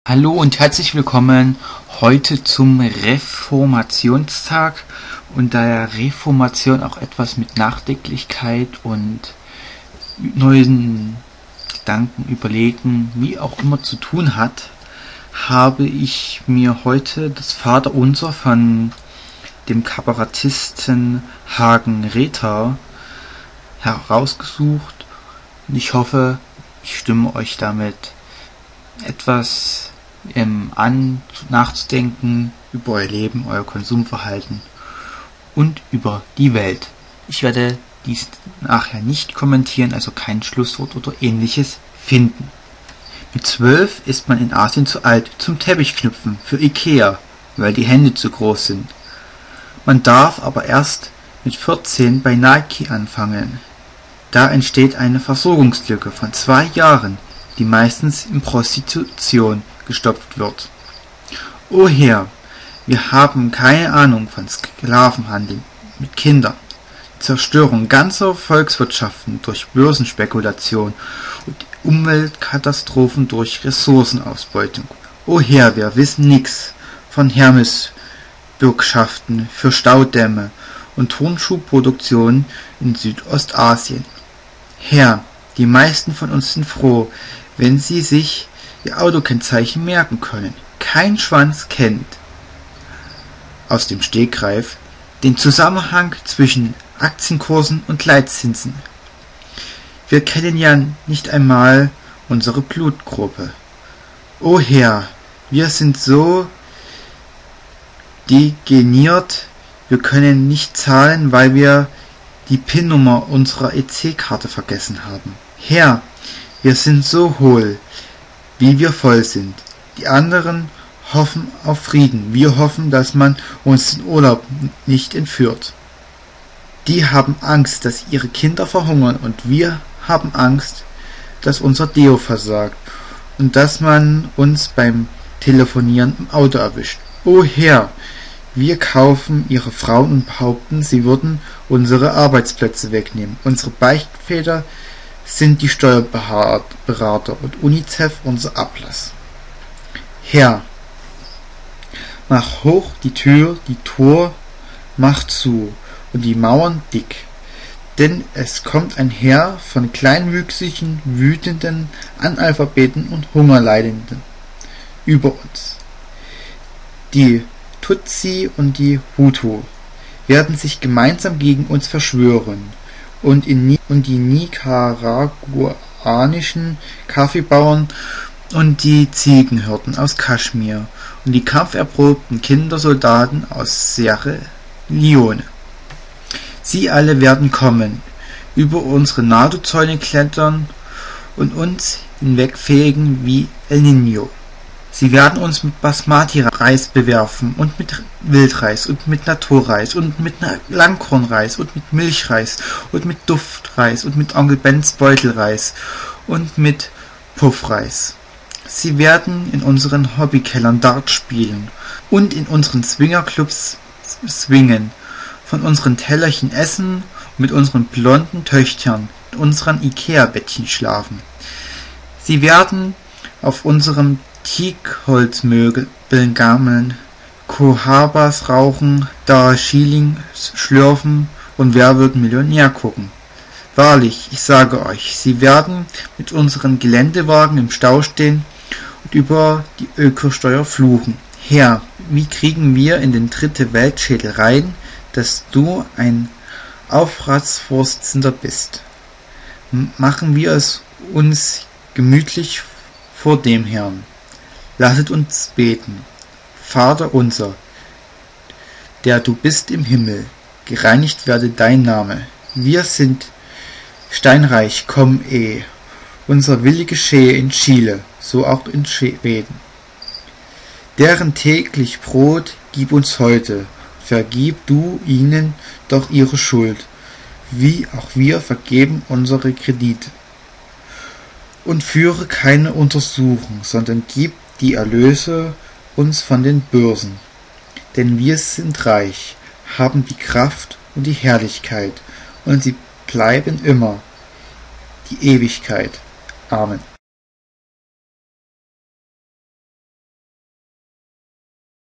Vater unser nach Hagen Rether [Deutsch]
Heute zur Feier des Reformationstages mal kein Bibeltext. Sehr bzw. hört euch alles bis zum Ende an! Die Intension bleibt euch überlassen. Wer eine Idee hat kann diese ruhig in Kommentaren festhalten.

Kommentiert, gelesen und produziert von Raphael Robertoson 2012